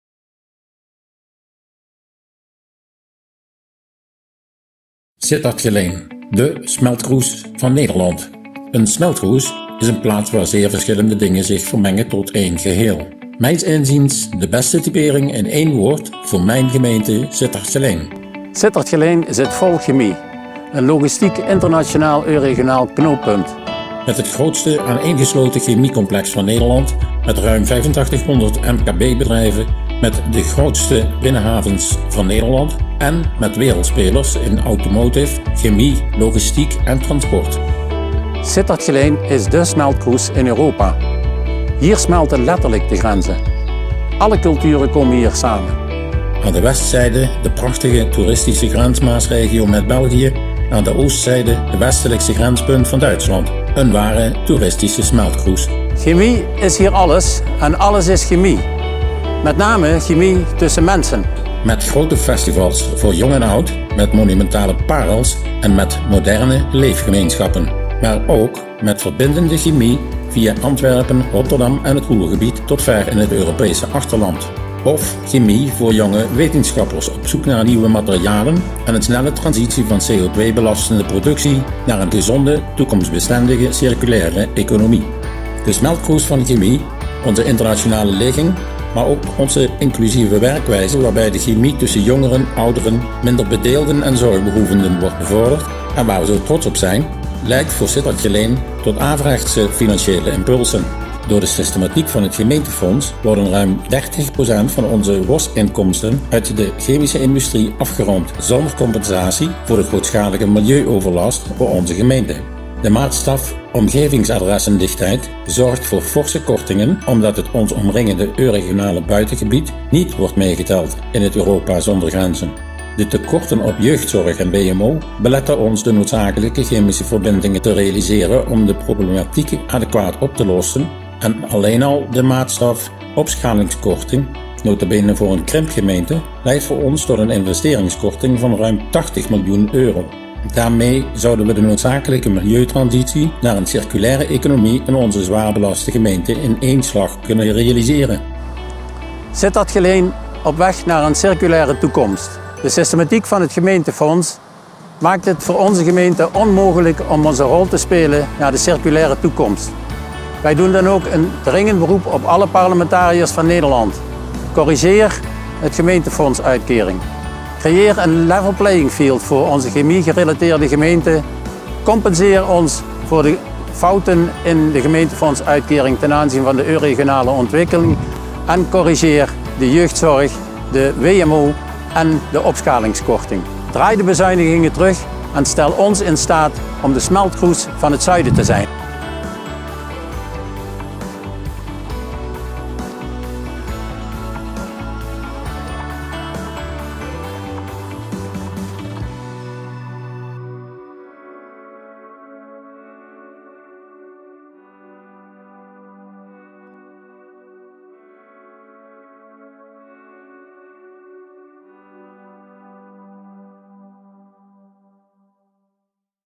Sittard-Geleen | Sittard-Geleen steunt protest
De gemeente Sittard-Geleen steunt de protestactie gemeenteninnood, die op 2 juli in Den Haag plaatsvindt.
Wethouders uit heel Nederland gaan naar Den Haag om aandacht te vragen voor de financiële problemen waar veel gemeenten mee te maken hebben.